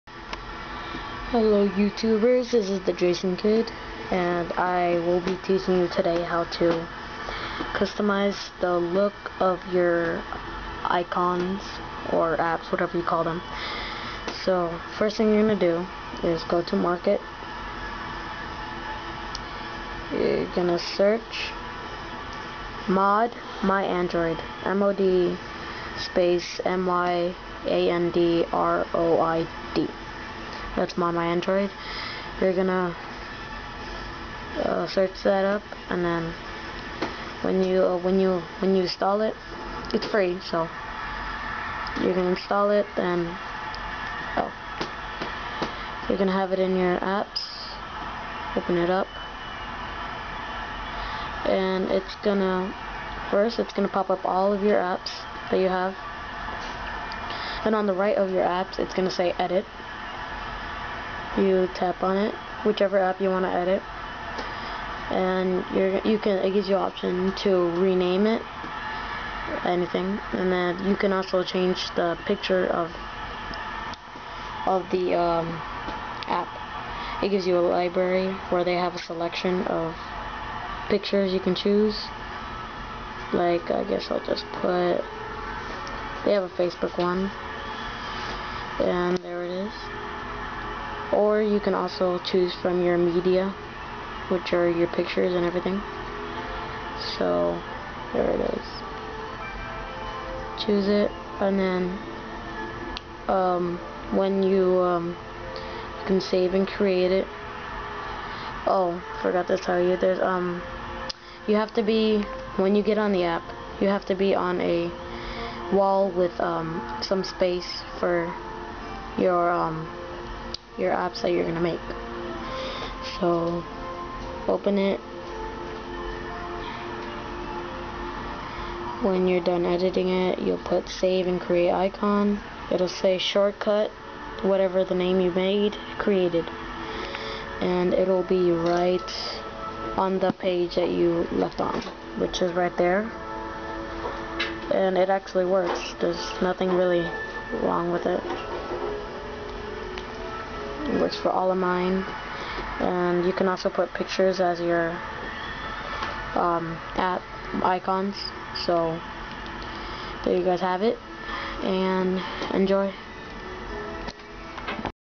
How to customize you're android phone Icons (MyTouch 3G)
Simple vid on how to customize the look of your icons i dont know if it work with any other androids but heres the MyTouch 3G